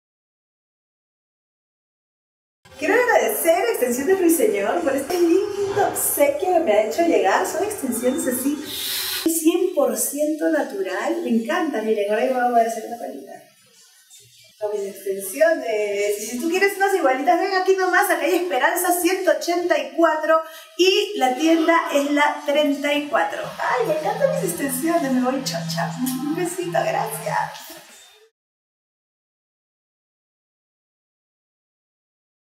Sandra Arana - Extensiones Ruiseñor
Extensiones Ruiseñor.
Cabello 100 % Natural.
Calidad Remy, recomendada por Sandra Arana.